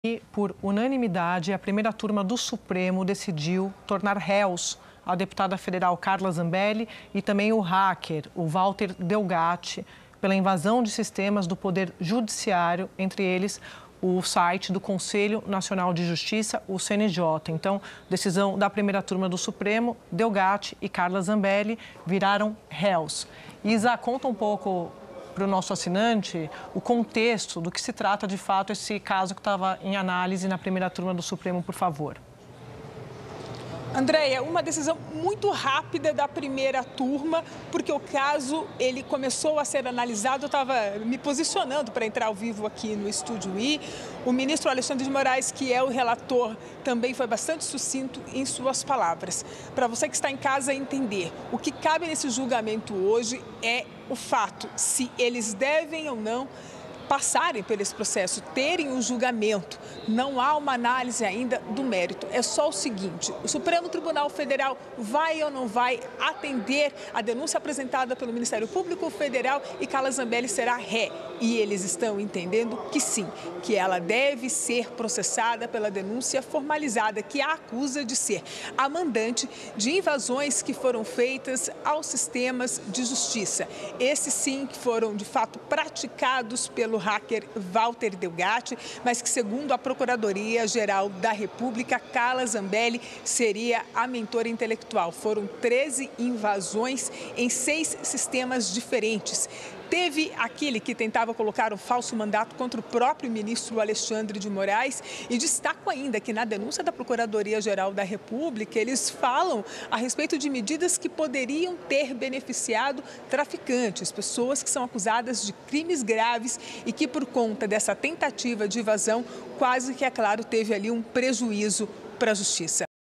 STF torna Carla Zambelli e Delgatti réus por invasão a sistemas do Judiciário
Olá!